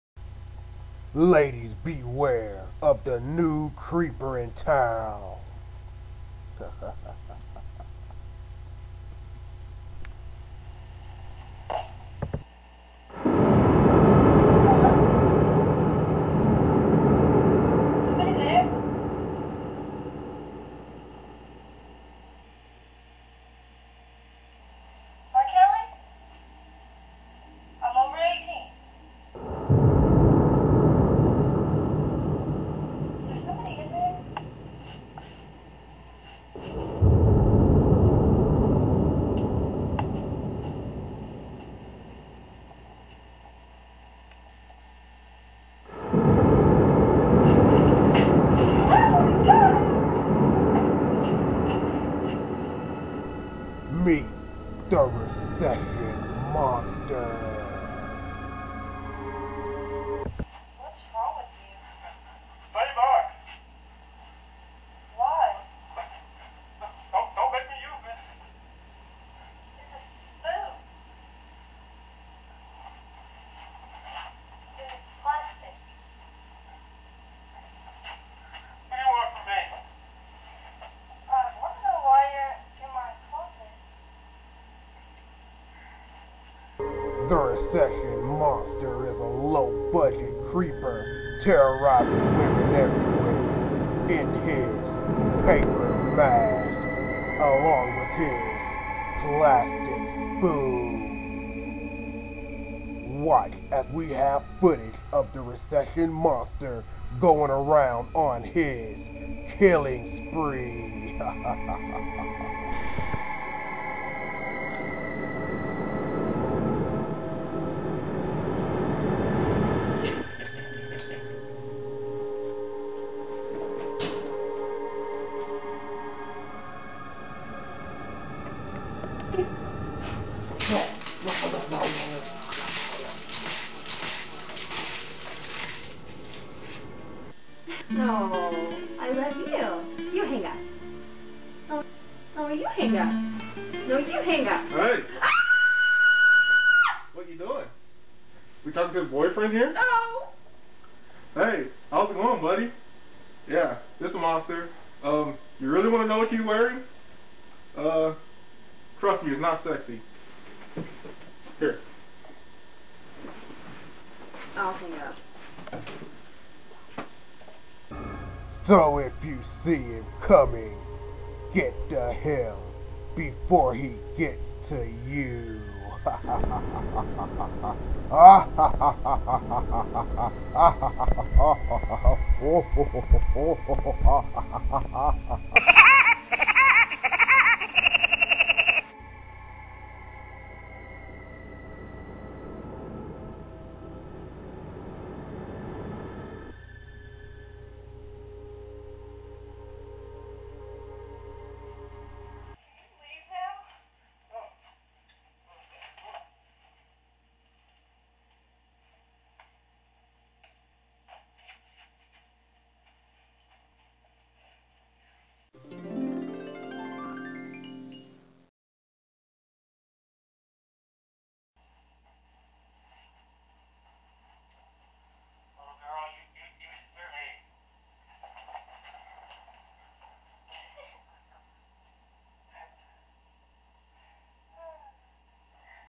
Recession Monster - Halloween TMD Video
Happy Halloween everyone. Here is a new, old holiday video from The Mighty Dynasty. Recession Monster is a low budget creeper who stalks women and tries to kill, Showing that even monsters need to make money. He may be more afraid of you than you are of him though. The video stars King Drell, Miss Amber, Miss Ashley, and Hardcore Bekie

TMD Bitches!!!